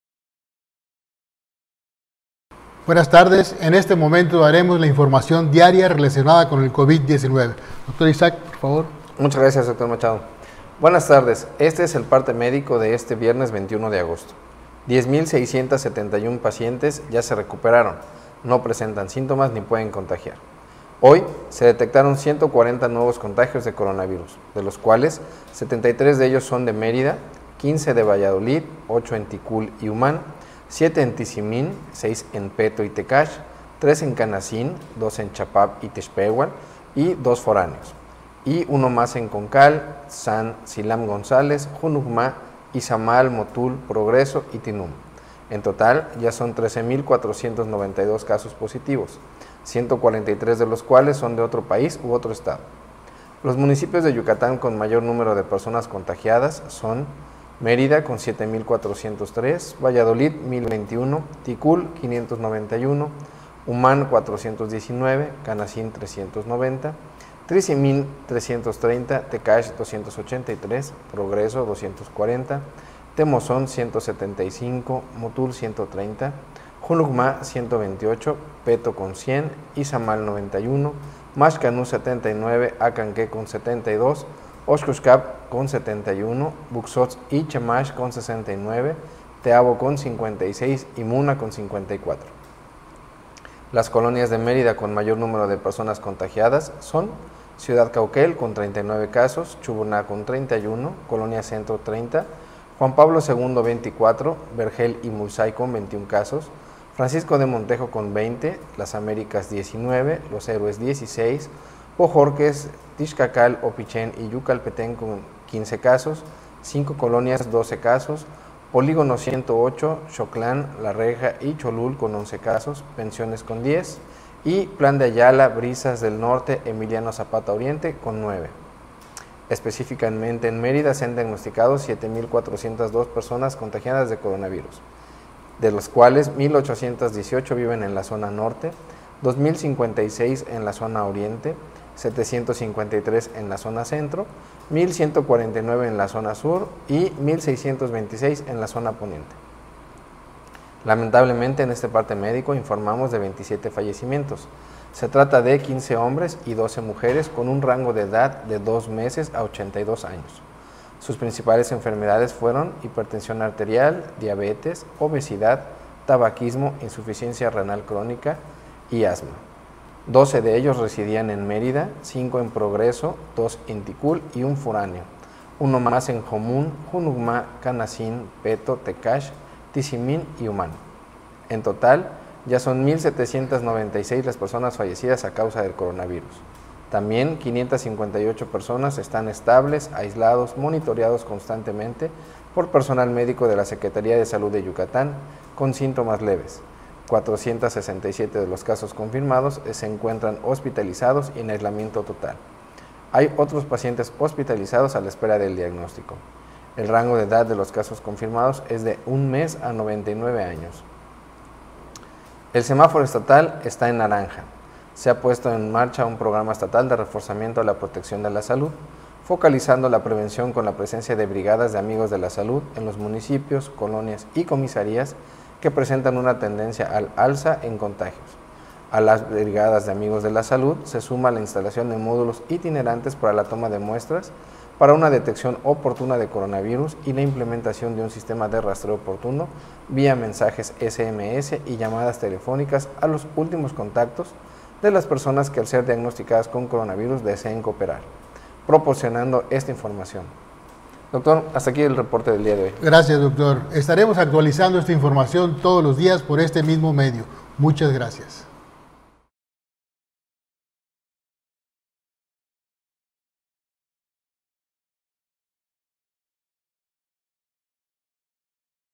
Informe diario del covid-19 en Yucatán, 21 de agosto de 2020
Hoy se detectaron 140 nuevos contagios de Coronavirus Lamentablemente se informan 27 fallecimientos:. Este es el parte médico de este viernes 21 de agosto:
10,671 pacientes ya se recuperaron: no presentan síntomas ni pueden contagiar.
Hoy se detectaron 140 nuevos contagios de Coronavirus.
73 en Mérida,
15 en Valladolid,
8 en Ticul y Umán,
7 en Tizimín,
6 en Peto y Tekax,
3 en Kanasín,
2 en Chapab y Tixpéual, y
2 foráneos, y
1 en Conkal, Dzan, Dzilam González, Hunucmá, Izamal, Motul, Progreso y Tinum.
En total, ya son 13,492 casos positivos, 143 de los cuales son de otro país u otro estado.
Lamentablemente, en este parte médico informamos de 27 fallecimientos: